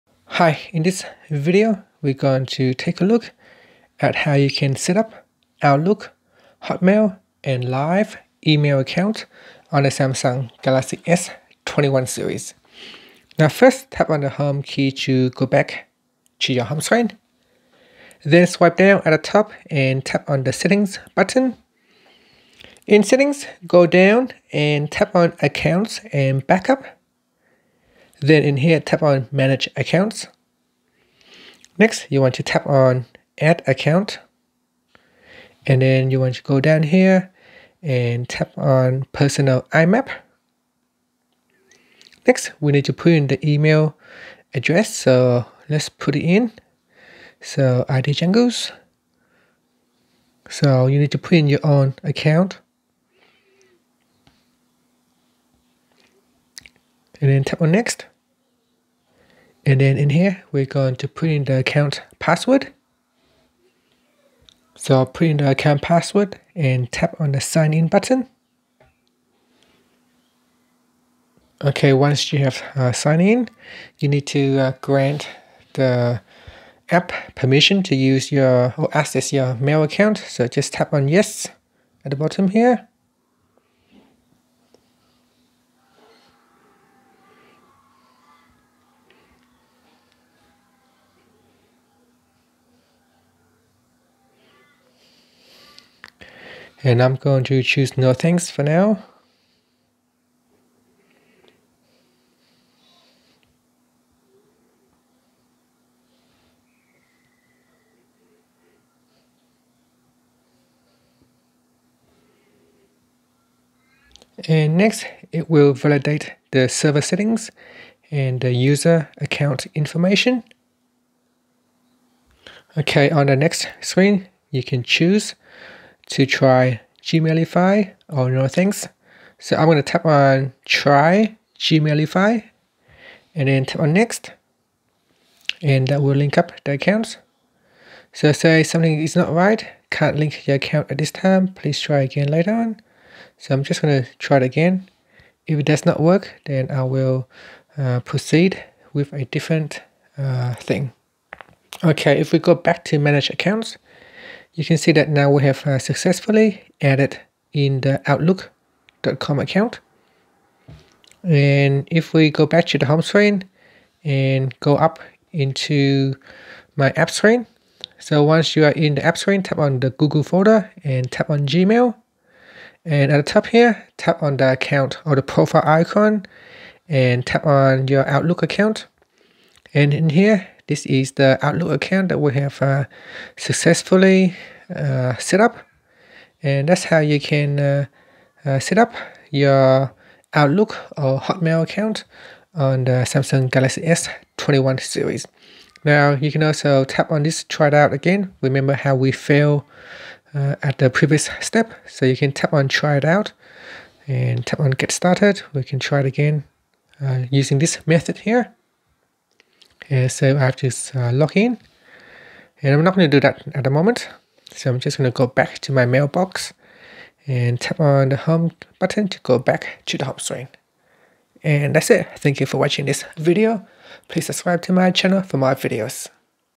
Galaxy S21/Ultra/Plus: How to Setup Outlook / Hotmail & Live Email Account
Learn how you can setup Outlook, Hotmail, and Live email account on the Galaxy S21/Ultra/Plus.